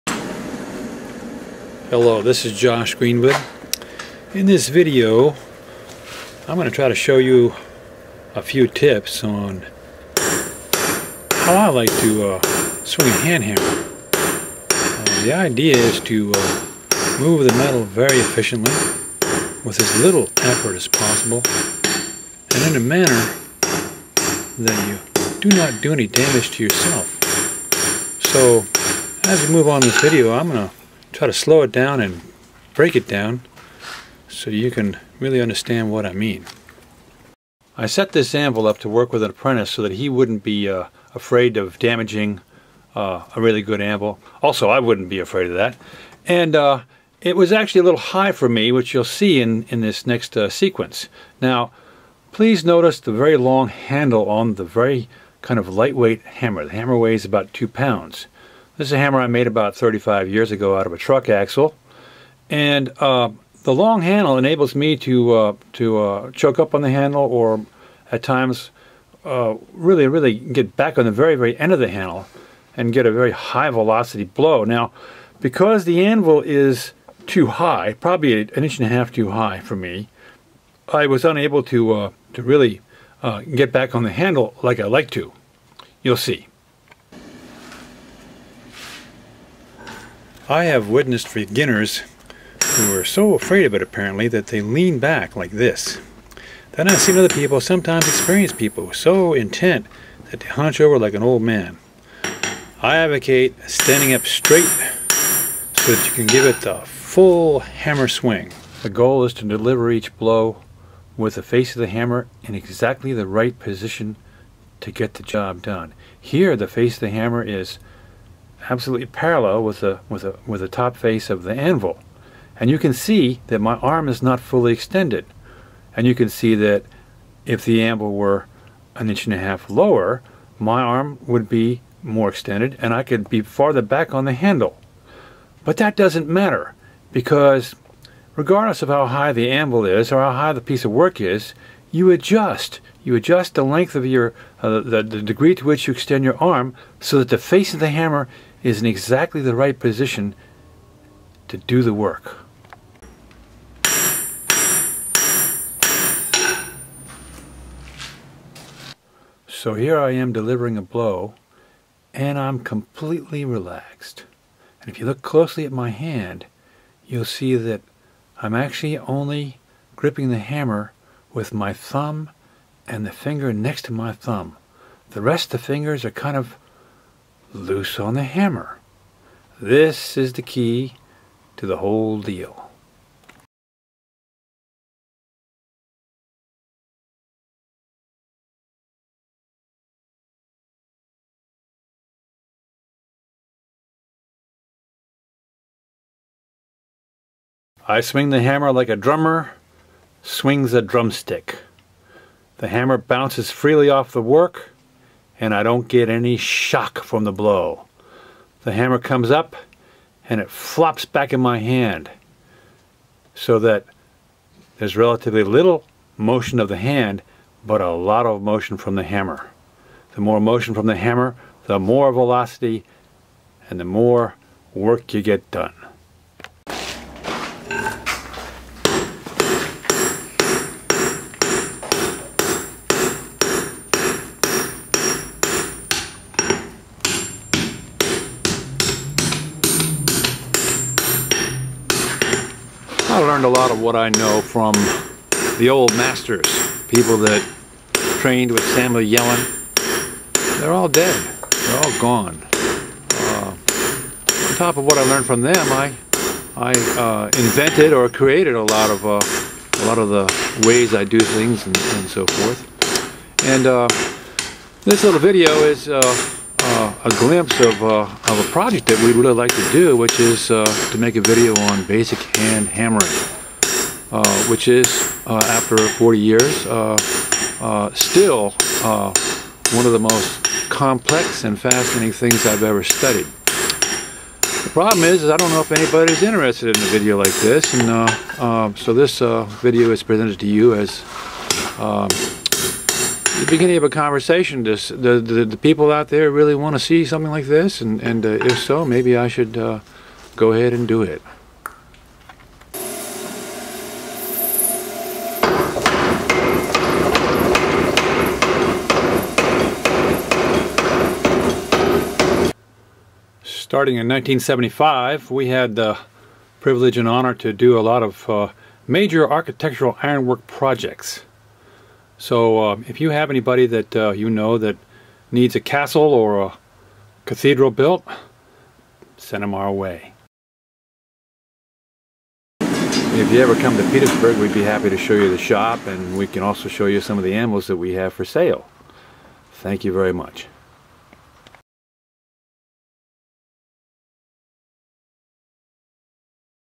Basic Hand Hammering
Every time a young blacksmith  comes to my shop to visit or purchase an anvil I show them how I swing a hammer with almost no effort, and without hurting myself. I have wanted to do a serious video about this for years, but its a big project and perhaps no one is interested.

 So here is my gift: a glimpse of this complex and fascinating subject. If you look closely at what I am doing you will see a lot going on that I don't explain.  I am using a light hammer with a very long handle, but the technique works just a well with a 3 or 4 pound hammer. In a pinch I will swing a sledge hammer with one hand - same technique!

There are some popular hand hammering theories out there that differ greatly from what I advocate: super heavy hammers, and short handled clubs, rocking torsos and hands above the head. The "thumb on top of the hammer" perfect control idea is ancient. A lot of beautiful work is done in ways that I have left behind or perhaps just don't understand.  I love you anyway.

If you get tired from hammering, or something hurts, please consider that there might be a better way. I remember going to my shop half exhausted one morning, but then I got into flow, and prepared and made 90 forge welds. 12 hours later I went home relaxed and refreshed.